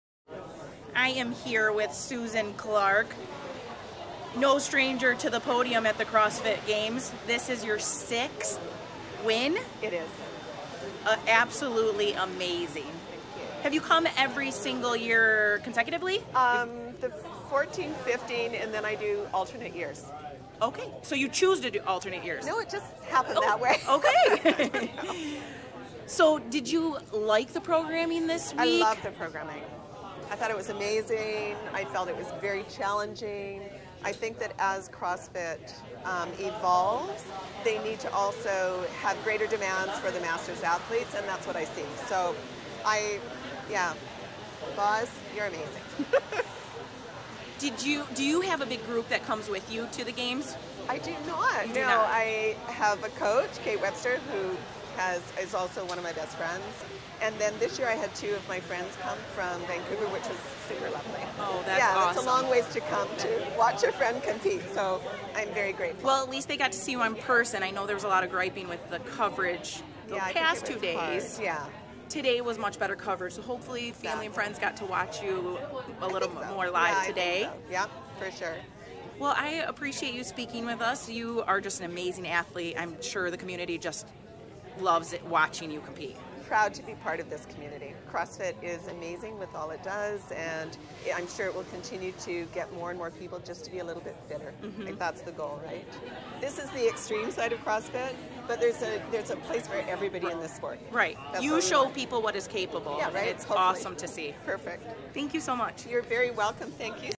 Susan Clarke 6 Time CrossFit Games Champion
Susan is not only an amazing champion, an extremely fit woman, but an inspiration to many showing what is possible